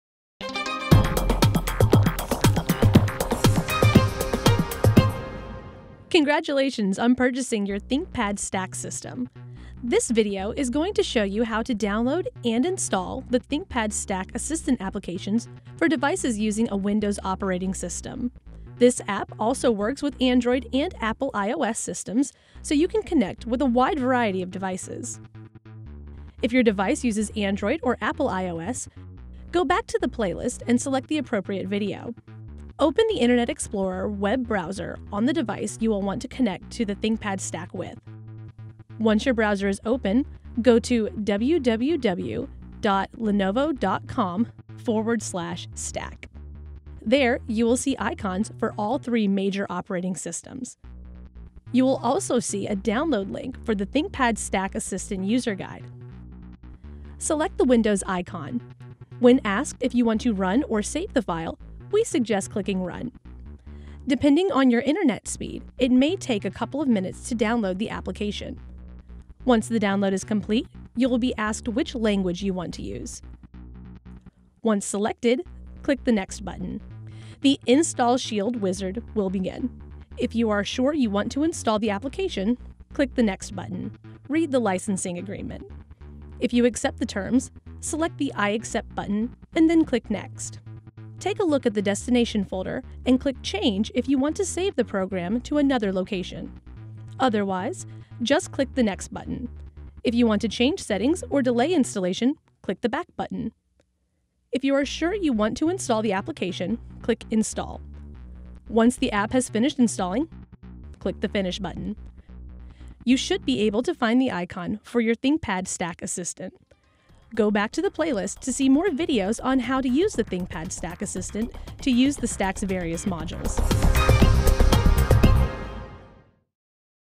How to install ThinkPad Stack Assistant for Windows
This how-to video will teach you the process for downloading and installing the Assistant app onto your Windows device. for more videos.
This essential software adds functionality and allows your Windows device to control your ThinkPad Stack system.